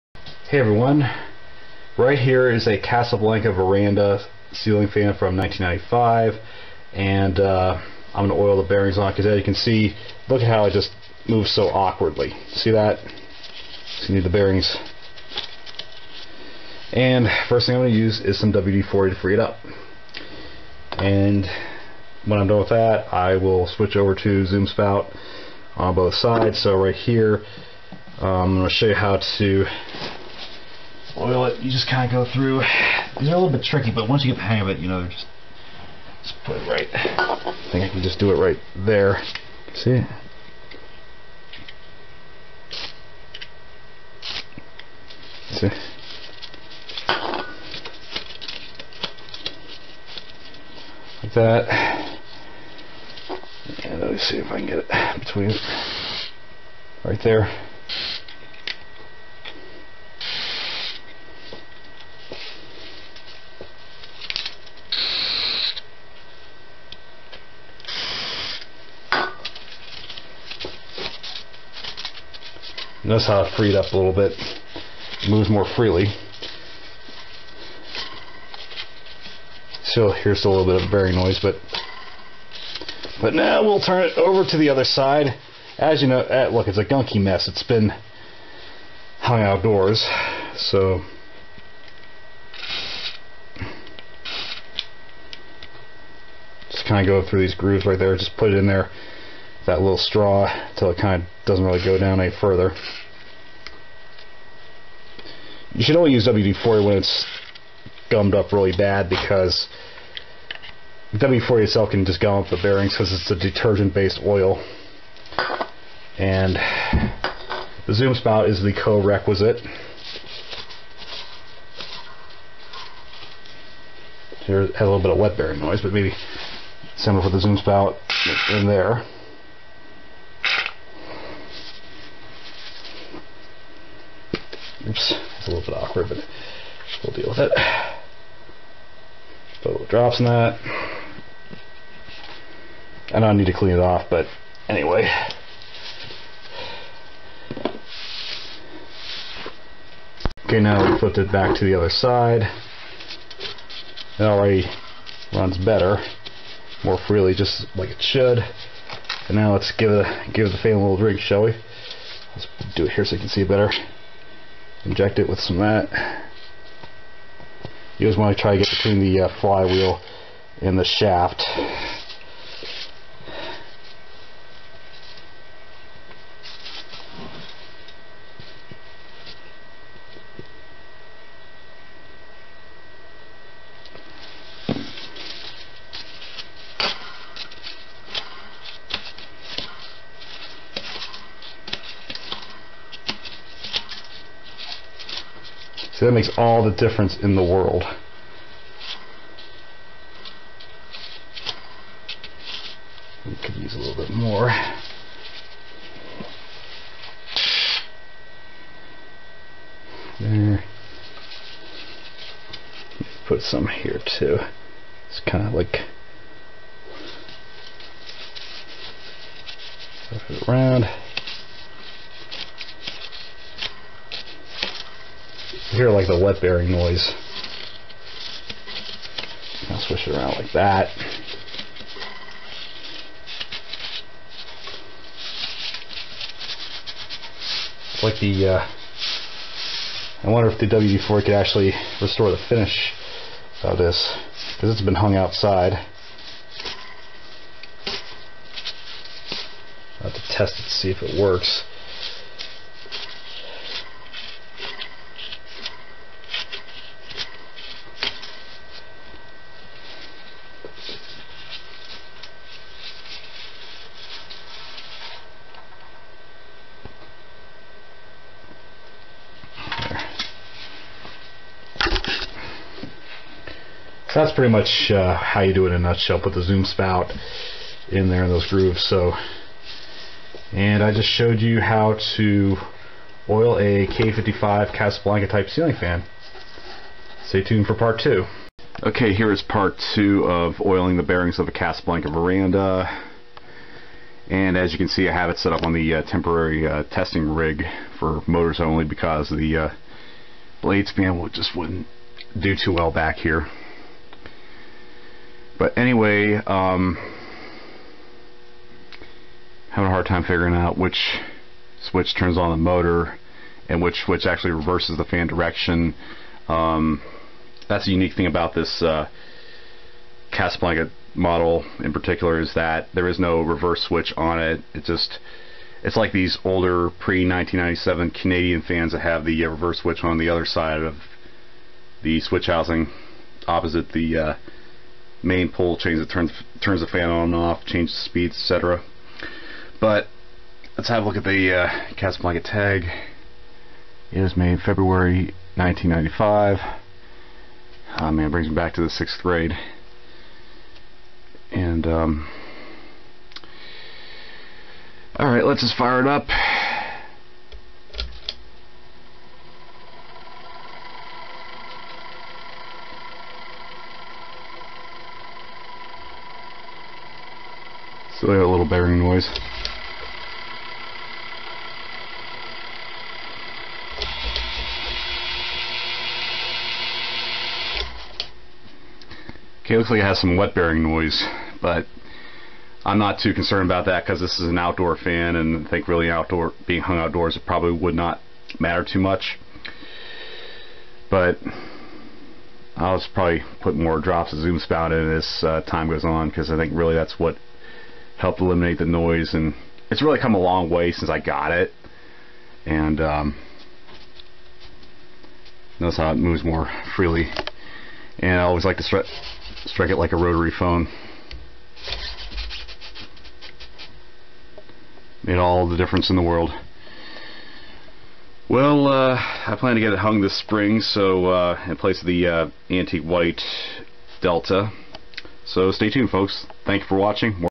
Oiling the bearings in a (K55) stack motor ceiling fan
Although there has already been a similar video posted regarding this topic, I will show you an in-depth method to oil bearings on a ceiling fan powered by a K55 stack motor. The featured fan is a Casablanca Veranda manufactured in February 1995, and is essentially the outdoor rated version of the much more common Delta II model.

Note: If you are to use WD-40 on a ceiling fan's bearings, please make sure you have some Zoom spout oil handy as WD-40 alone can gum up bearings as it is a detergent based oil. WD-40 should only be used in initially cleaning the dirty bearings.